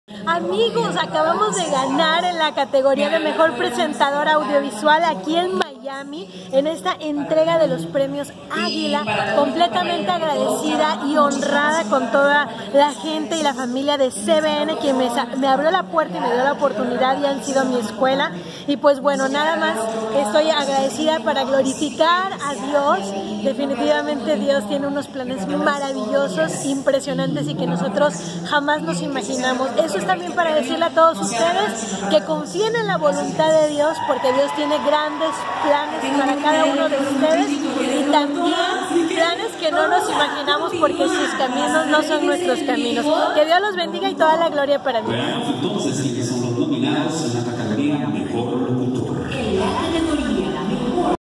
NADIA AGRADECE PREMIO AGUILA
Nadia gana importante premio internacional PREMIO AGUILA , como mejor presentadora audiovisual en el programa VIVE + que se transmite en 17 países y en mejor por tv abierta de Azteca 7.2